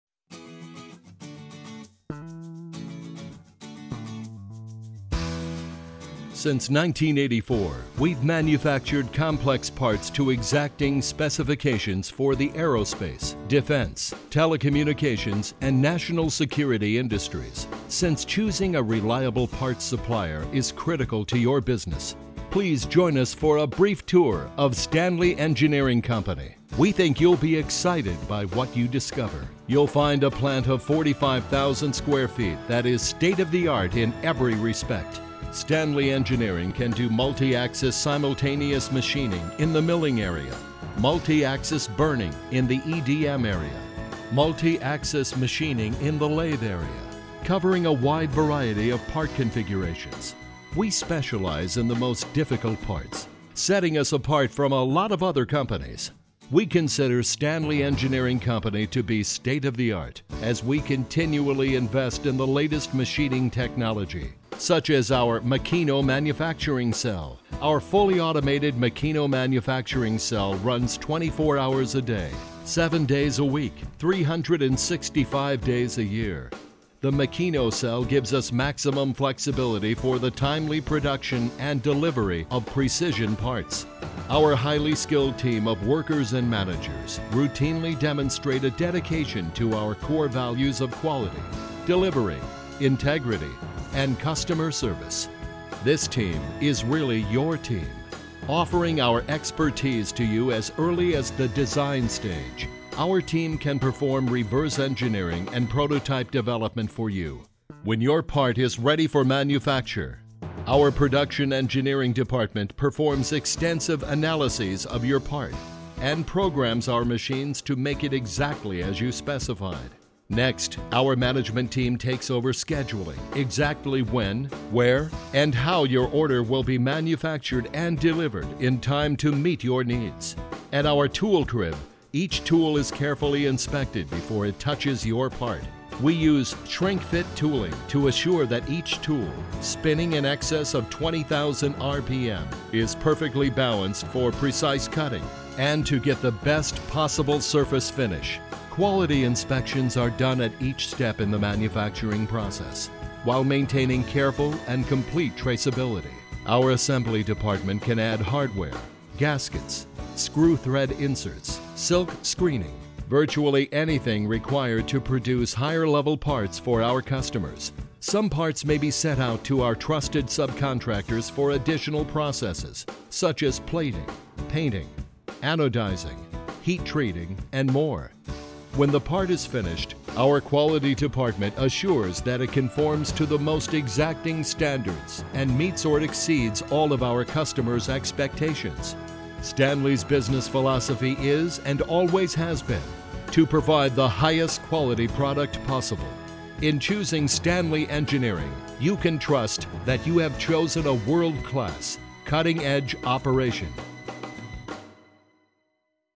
Stanley Engineering - Company Overview 2019
Since 1984 Stanley Engineering has manufactured complex parts to exacting specifications for the Aerospace, Defense, Telecommunications, and National Security industries.
Making precision parts like these, for companies like yours, is our pride our passion and core business at Stanley engineering company.
Our company overview video provides you a view into our facility. Thank you for visiting.